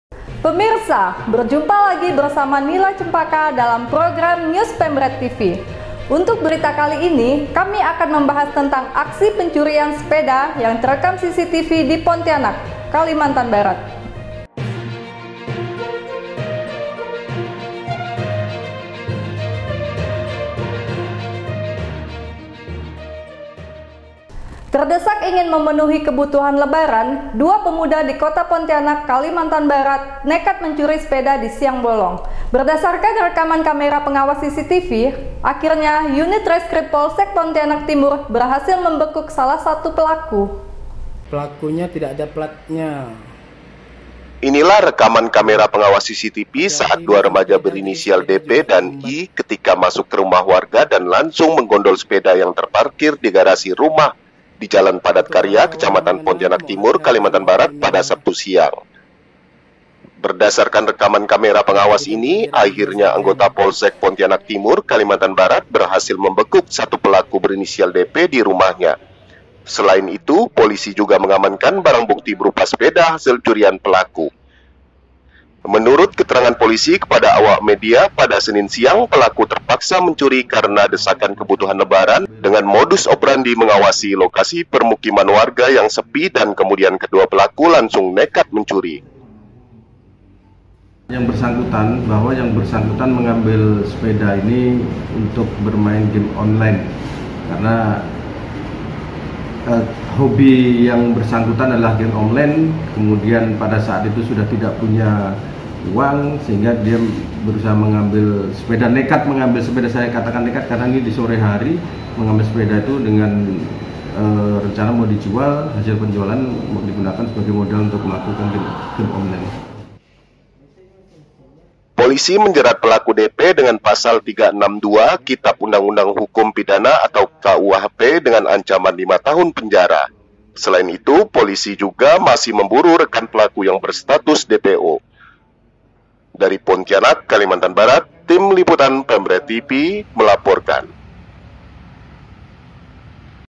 Terekam CCTV Saat Mencuri Sepeda, Pemuda Tanggung Dibekuk Polisi di Pontianak
PONTIANAK, SP - Terdesak ingin memenuhi kebutuhan lebaran,  2 pemuda di Kota Pontianak, Kalimantan Barat nekat mencuri sepeda di siang bolong.  Berdasarkan rekaman kamera pengawas CCTV,  akhirnya unit reskrim Polsek Pontianak Timur berhasil membekuk salah satu pelaku pencurian.

Dalam tayangan PemredTV diatas, menayangkan rekaman kamera pengawas CCTV saat dua remaja berinisial dp dan i menerobos masuk ke salah satu rumah warga, dan langsung menggondol sepeda yang terparkir di garasi rumah di Jalan Padat Karya, Kecamatan Pontianak Timur pada Sabtu (01/8) siang beberapa waktu lalu.

Berdasarkan rekaman kamera pengawas ini,  akhirnya Anggota Polsek Pontianak Timur berhasil membekuk satu pelaku berinisial dp di rumahnya.  Selain itu, polisi juga mengamankan barang bukti berupa sepeda hasil curian pelaku.

Menurut keterangan Kapolsek Pontianak Timur AKP Prayitno,  Senin (03/8) siang,  pelaku terpaksa mencuri karena desakan kebutuhan lebaran. Dengan modus operandi mengawasi lokasi permukiman warga yang sepi,  kemudian kedua pelaku langsung nekat mencuri.

Akibat perbuatannya, pelaku dp dijerat dengan pasal 362 Kitab Undang Undang  Hukum Pidana (KUHP) dengan ancaman hukuman 5 tahun penjara. Selain itu, polisi juga masih memburu rekan pelaku  yang berstatus sebagai  DPO.

saksikan berita selengkapnya, dalam tayangan News PemredTV diatas.